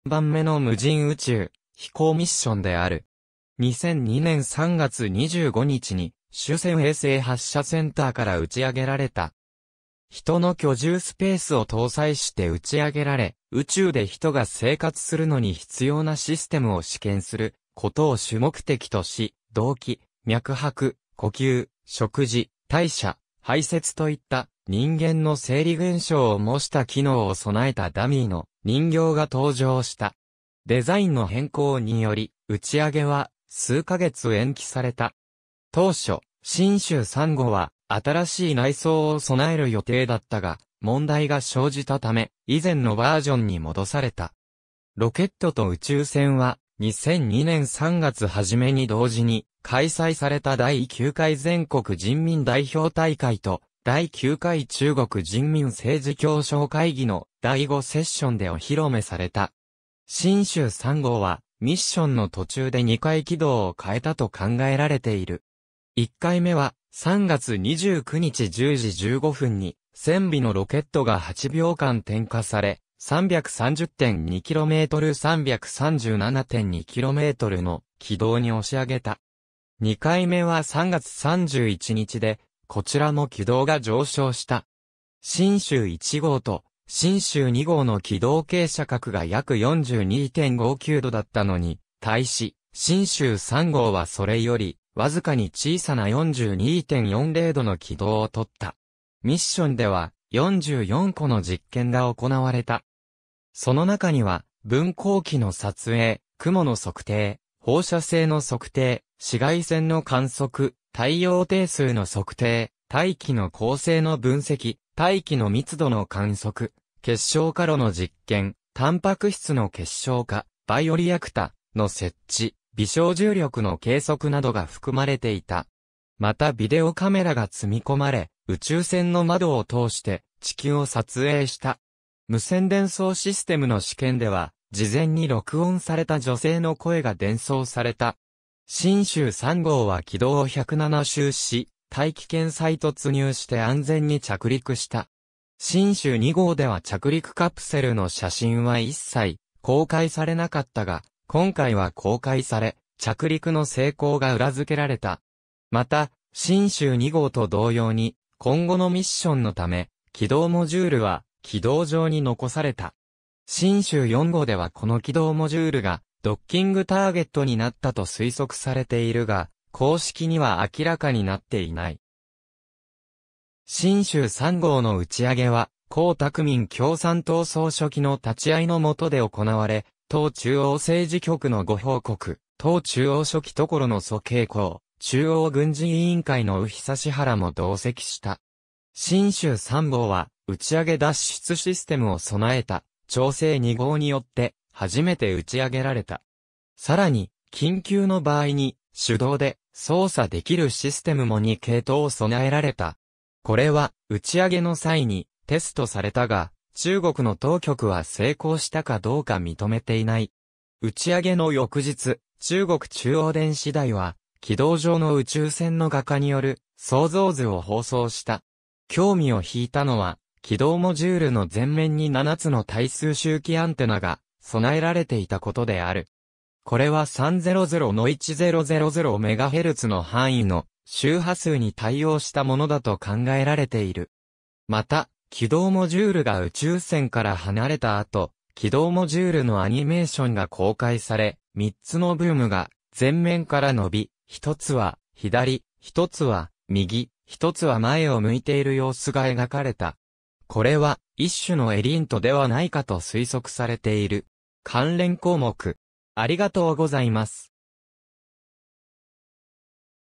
神舟3号
神舟3号（Shenzhou 3、中国語:神舟三號）は、中華人民共和国の3番目の無人宇宙飛行ミッションである。
2002年3月25日に酒泉衛星発射センターから打ち上げられた。
人の居住スペースを搭載して打ち上げられ、宇宙で人が生活するのに必要なシステムを試験することを主目的とし、動悸、脈拍、呼吸、食事、代謝、排泄といった、人間の生理現象を模した機能を備えたダミーの人形が搭乗した。
デザインの変更により、打ち上げは数か月延期された。
当初、神舟3号は新しい内装を備える予定だったが、問題が生じたため、以前のバージョンに戻された。
ロケットと宇宙船は、2002年3月初めに同時に開催された第9回全国人民代表大会と第9回中国人民政治協商会議の第5セッションでお披露目された。
神舟3号は、ミッションの途中で2回軌道を変えたと考えられている。
1回目は、3月29日10時15分（UTC）に船尾のロケットが8秒間点火され、330.2km×337.2kmの軌道に押し上げた。
2回目は3月31日で、こちらも軌道が上昇した。
神舟1号と神舟2号の軌道傾斜角が約42.59°だったのに対し、神舟3号はそれよりわずかに小さな42.40°の軌道をとった。
ミッションでは、44個の実験が行われた。
その中には、分光器の撮影、雲の測定、放射性の測定、紫外線の観測、太陽定数の測定、大気の構成の分析、大気の密度の観測、結晶化炉の実験、タンパク質の結晶化、バイオリアクタ―の設置、微小重力の計測などが含まれていた。
またビデオカメラが積み込まれ、宇宙船の窓を通して地球を撮影した。
無線伝送システムの試験では、事前に録音された女性の声が伝送された。
神舟3号は軌道を107周し、大気圏再突入して安全に着陸した。
神舟2号では着陸カプセルの写真は一切公開されなかったが、今回は公開され、着陸の成功が裏付けられた。
また神舟2号と同様に、今後のミッションのため、軌道モジュールは軌道上に残された。
神舟4号ではこの軌道モジュールがドッキングターゲットになったと推測されているが、公式には明らかになっていない。
神舟3号の打ち上げは、江沢民共産党総書記の立ち会いの元で行われ、党中央政治局の呉邦国、党中央書記処の曽慶紅、中央軍事委員会の于永波らも同席した。
神舟3号は、打ち上げ脱出システムを備えた長征2号によって初めて打ち上げられた。
さらに、緊急の場合に手動で操作できるシステムも2系統供えられた。
これは打ち上げの際にテストされたが、中国の当局は成功したかどうか認めていない。
打ち上げの翌日、中国中央電視台は軌道上の宇宙船の画家による想像図を放送した。
興味を引いたのは、軌道モジュールの前面に7つの対数周期アンテナが備えられていたことである。
これは300-1000MHzの範囲の周波数に対応したものだと考えられている。
また、軌道モジュールが宇宙船から離れた後、軌道モジュールのアニメーションが公開され、3つのブームが前面から伸び、1つは左、1つは右、1つは前を向いている様子が描かれた。
これは、一種のELINTではないかと推測されている。
関連項目